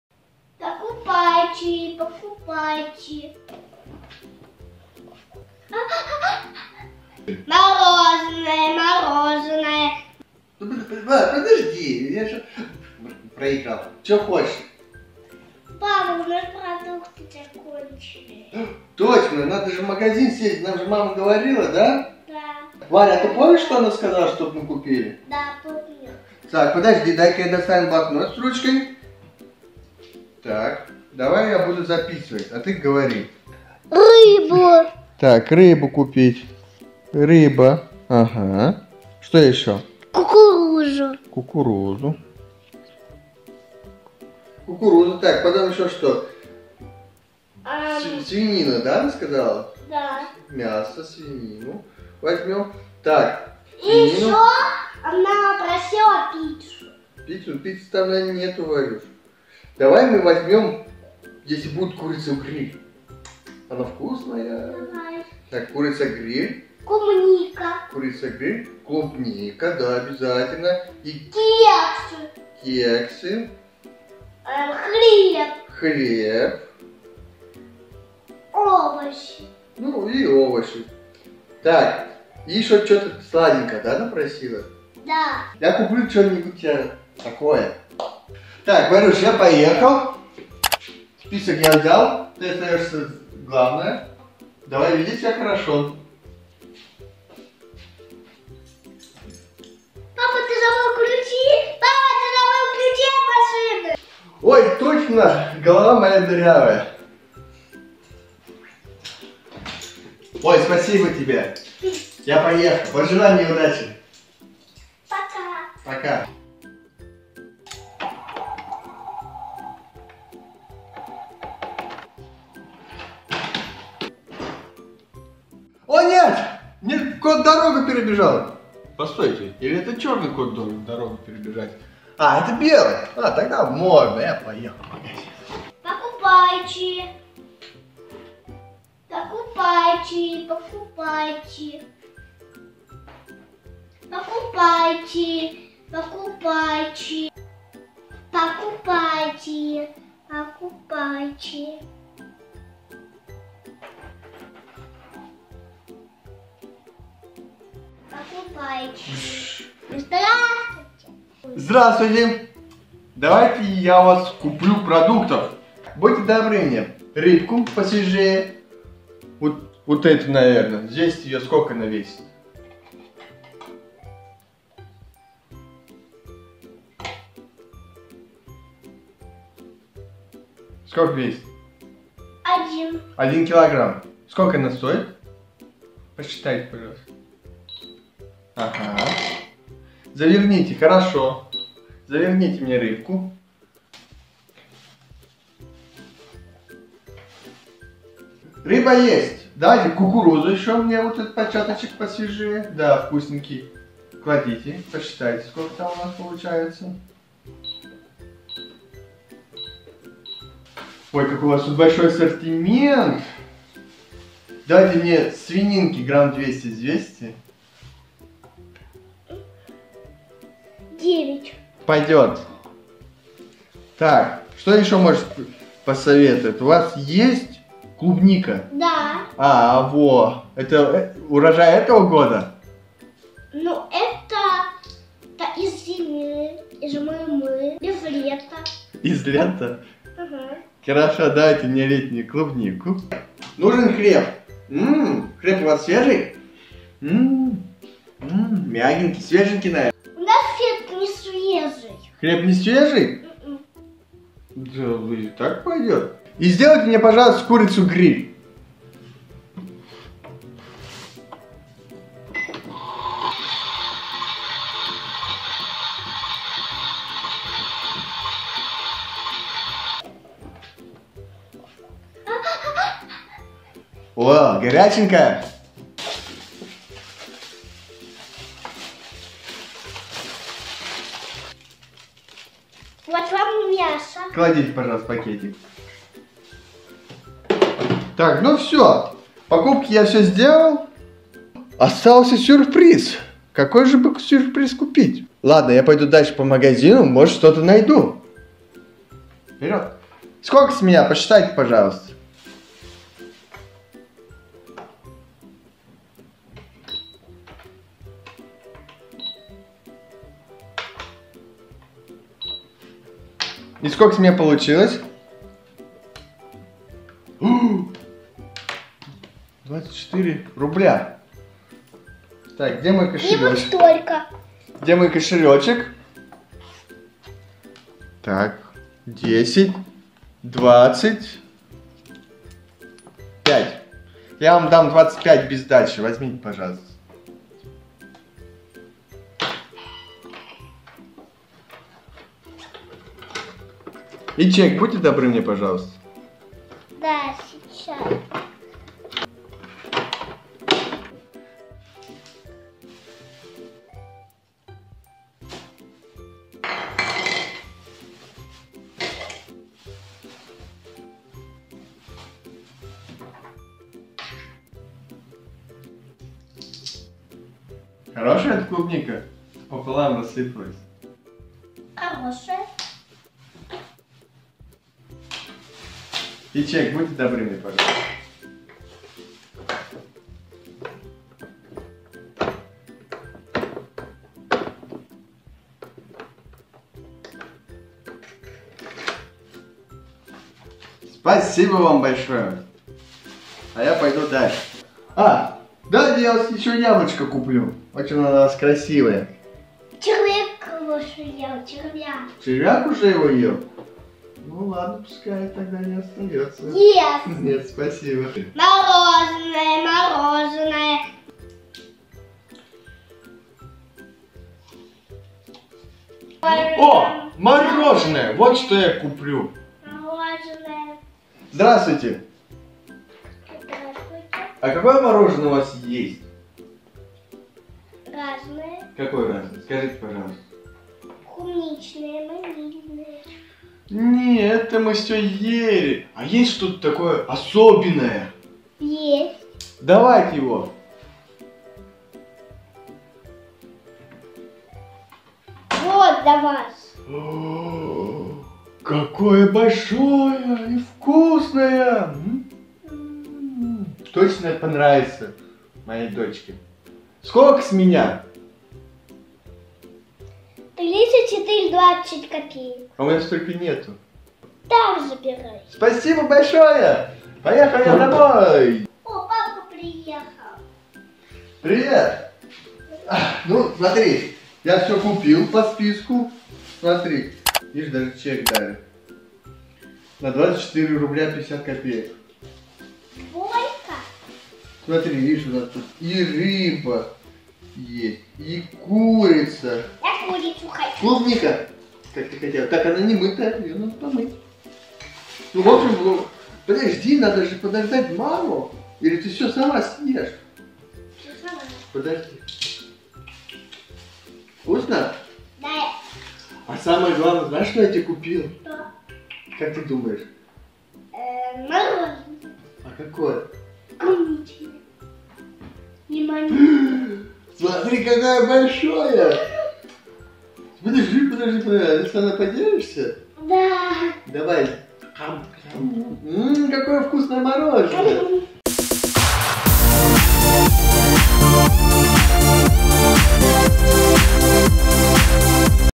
Варя и папа играют в магазин Детское видео pretend play
pretend play Играем вместе с папой в продавца мороженого и продуктов. Варя весёлый продавец и папа смешной покупатель играют в магазин. У Вари даже есть своя игрушечная касса)